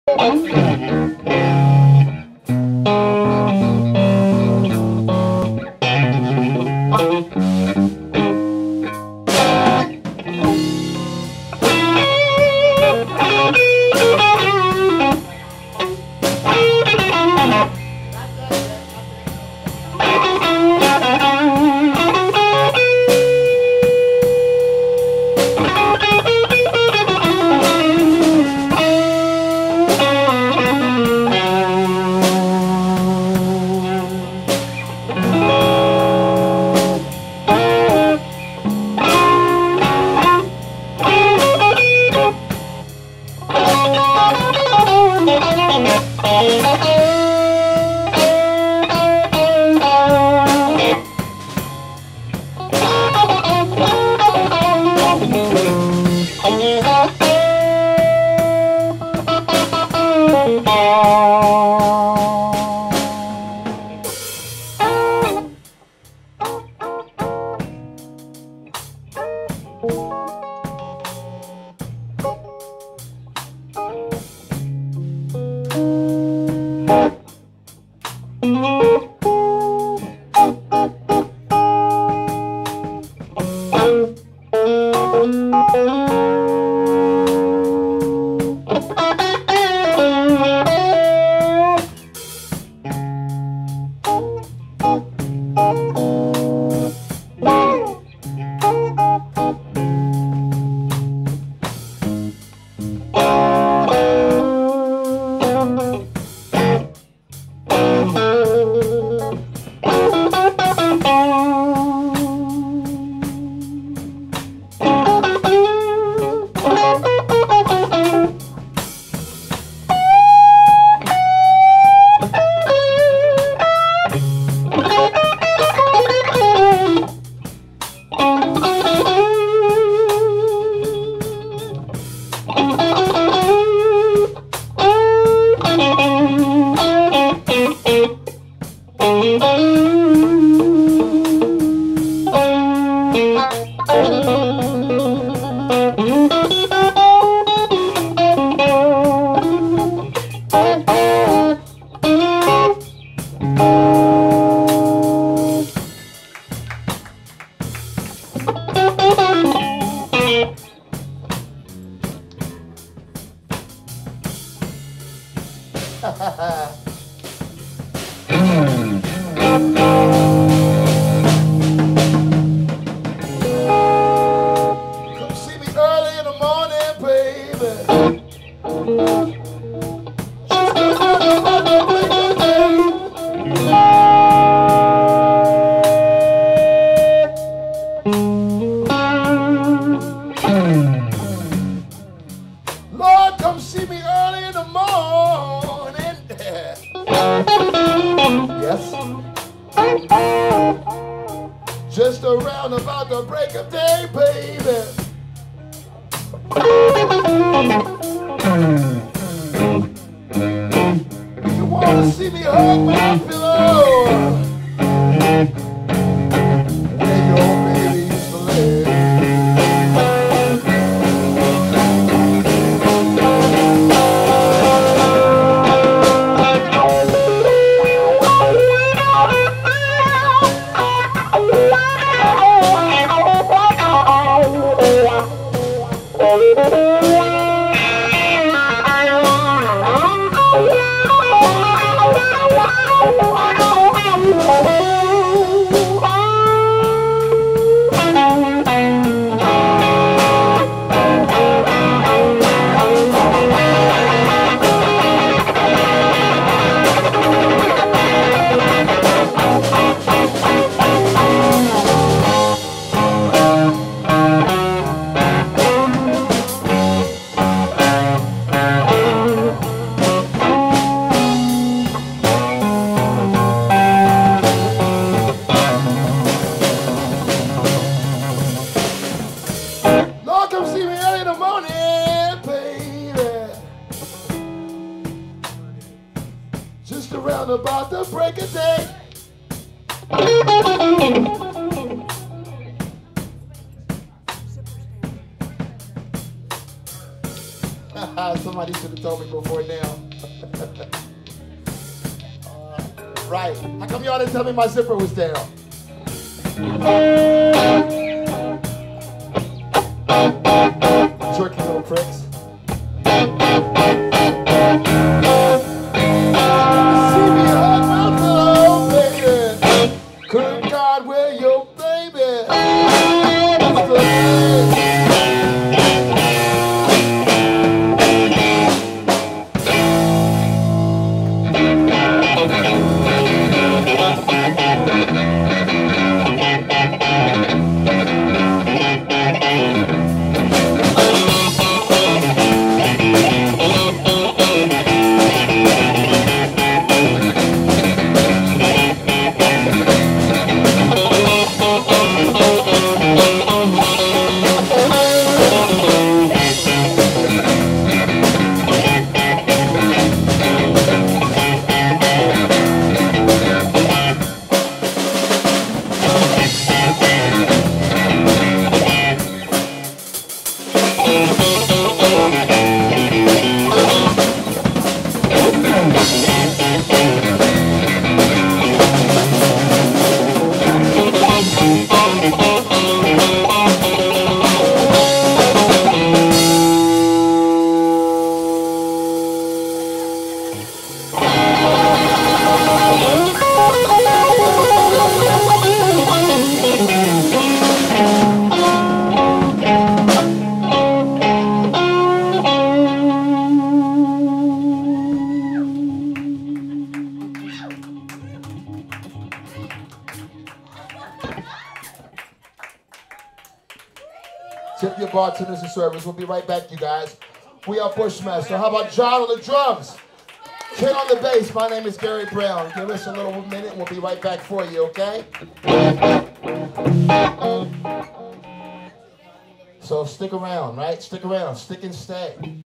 011 Bushmaster at 551 West 2018-10-20
Bushmaster featuring Gary Brown performed LIVE at 551 West in Lancaster, PA, on Saturday 10/20/18.  Thanks go out to the awesome and welcoming staff, the fun folks who came out to support live music, and special thanks to fellow musician Octavia for hooking us up with drummer John Whiteley at the last minute... he played like he's been in the band for months and we really had a blast!  Here's the video footage from the night to share with you... captured on the band's ZoomQ4N HD video camera, this song/playlist is perfect to cast to your favorite HD TV, bluetooth, or other HD device... let us know your thoughts... We LOVE feedback ;-)

Ken Fiester - bass.
John Whiteley - drums.

...more about the band at BushmasterBlues.com